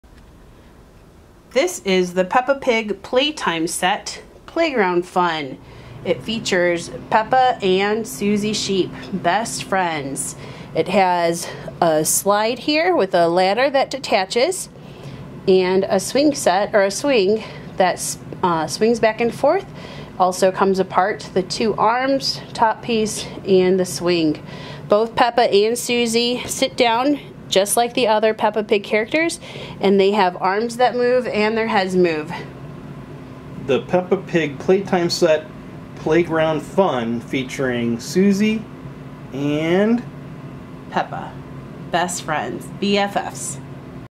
Review of Peppa Pig Playtime Set, Playground Fun
This Peppa Pig Playtime Set includes Peppa Pig, Suzie Sheep, a slide, and a swing. The characters fit on both playground equipment.